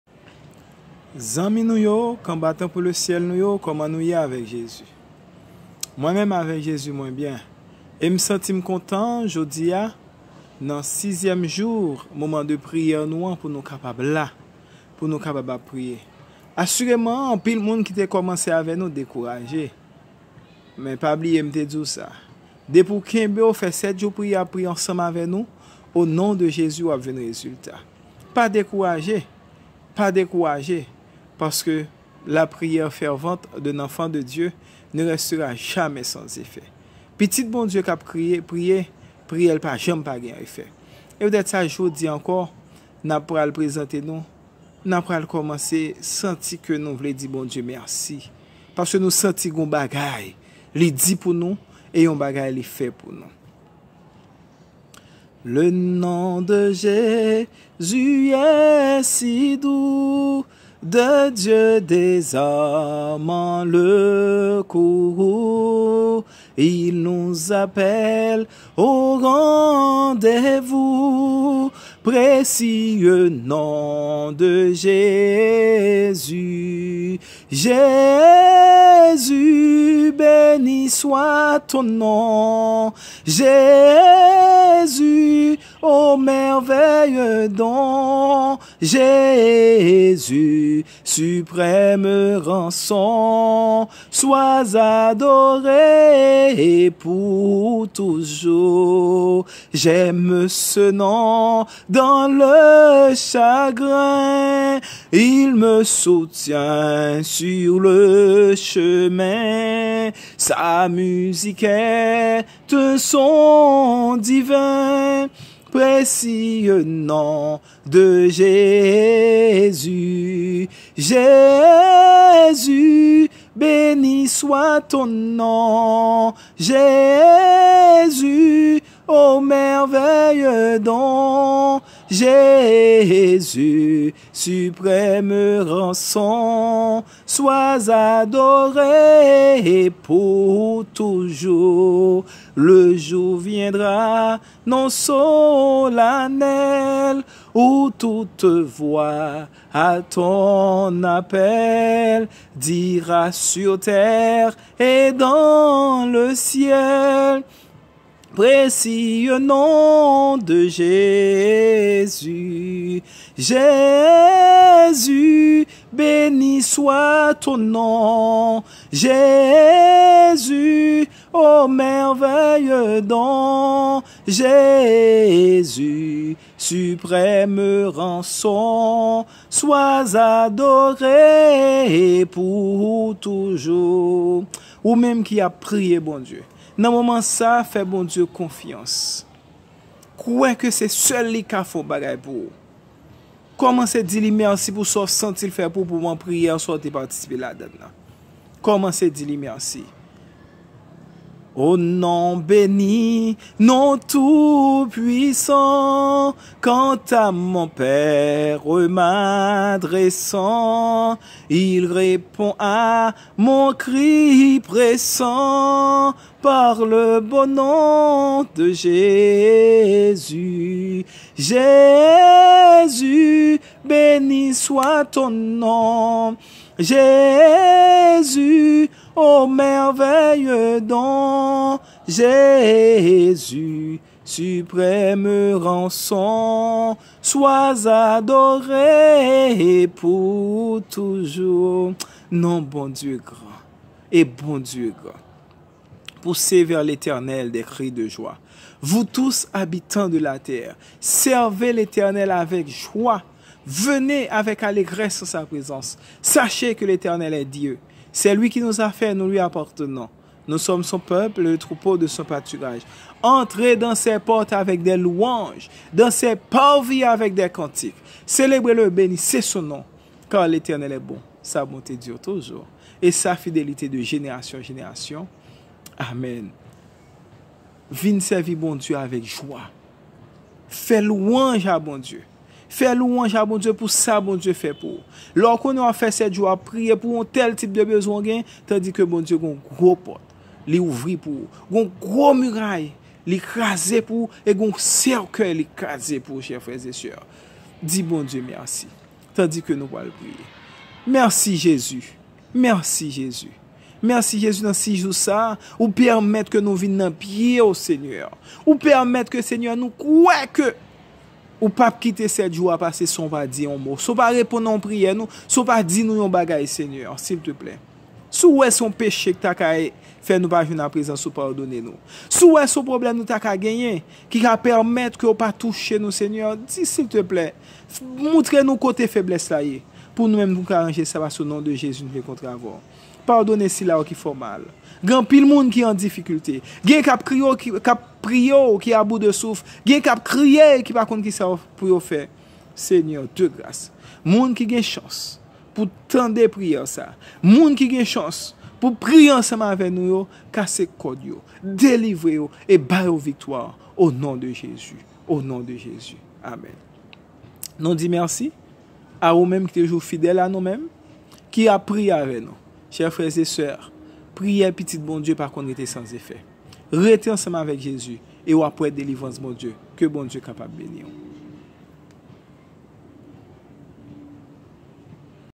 6èmejou la priyè .Bondye pa komanse poul pa fini.
Nan seri sa nou komanse di Bondye mesi pou sal komanse fe nan dosye nou.
Tou moun ki fe priyè sa ap komanse delivre.